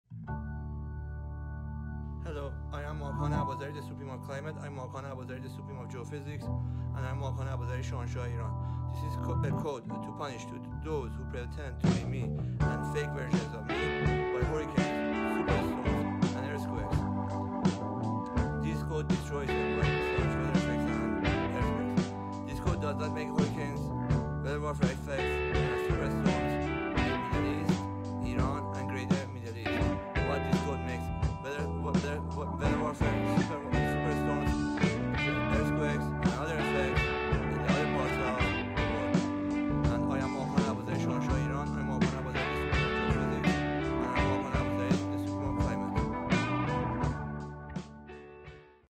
Superstorms and Earthquakes to punish fake versions of Makan Abazari
This code works by entering it on Social Media or text messaging it. If Islamic Republic of Iran qualify to enter the code they can also do the same. It is better to build a climate control facility in Iran to control climate of all of the world. The Climate Control Facility will be under my direct leadership. Creating several organizations in Iran to enter the code creates long term security for everyone. Until Iran builds the climate control center Iran can enter the code on Social Media or Text Messaging it. It can be text messaged to 113 Ettelat Intelligence agency of Iran or other phone numbers chosen by Iran. I am Makan Abazari.  On this twitter video you can see an explanation of the system